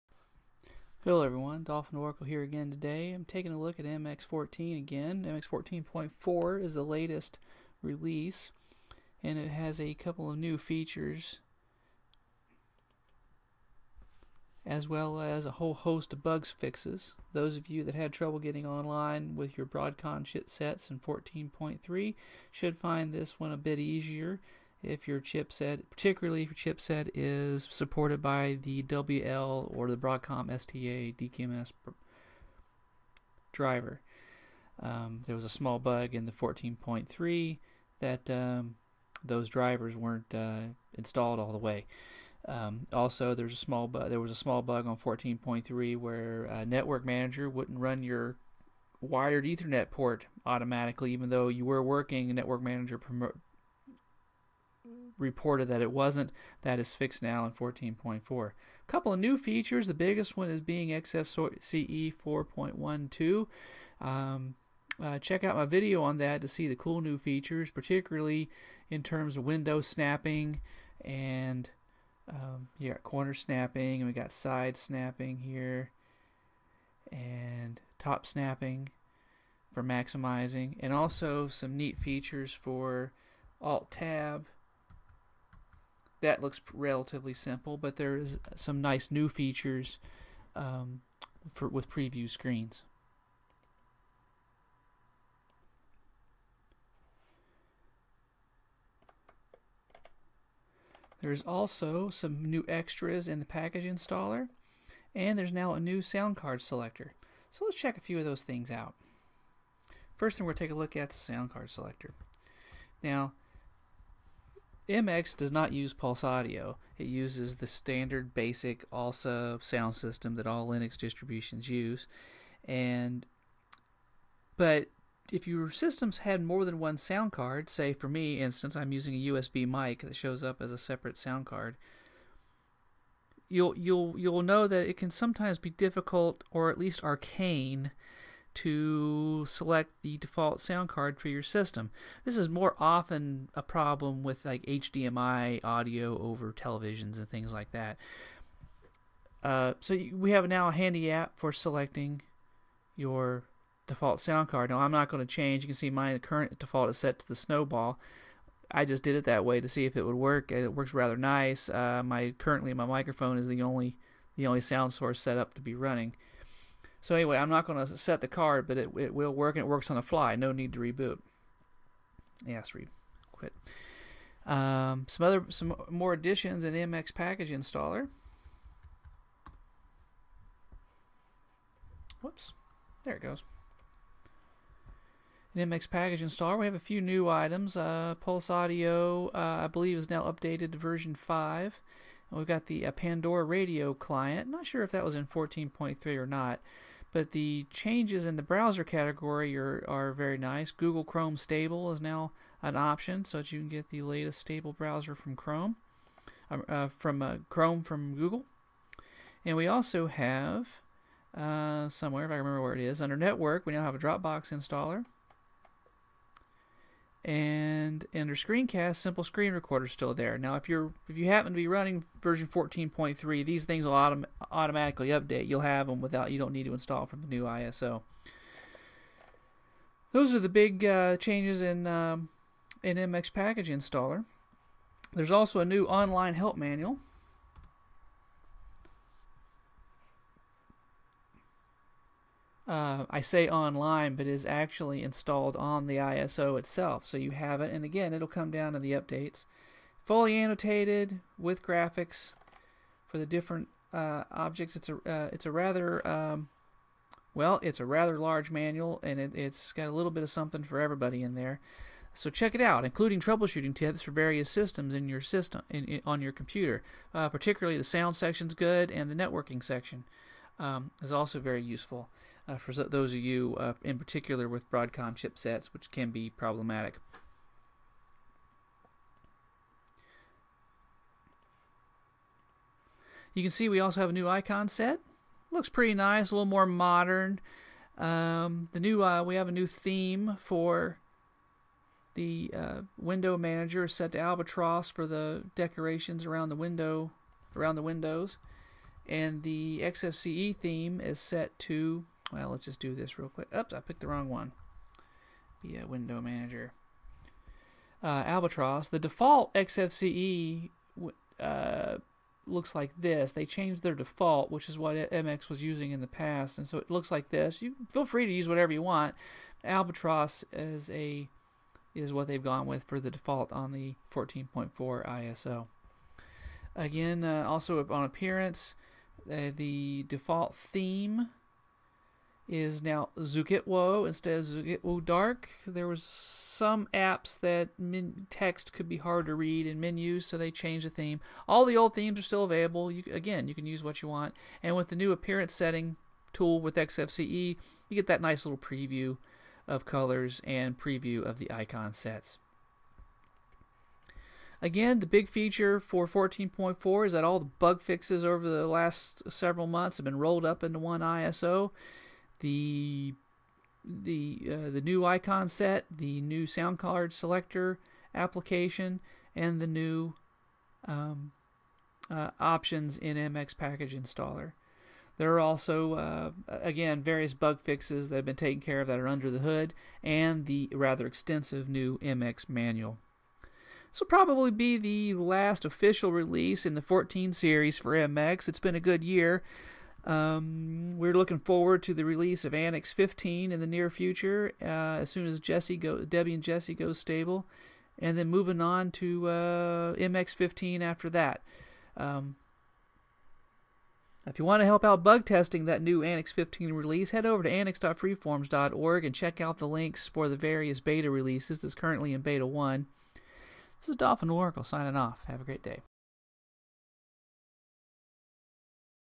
MX 14.4 w/ XFCE 4.12 Upgrade & Bugflix Release
Checking out some new features in MX 14.4 Upgrade & Bugfix release.

d.o.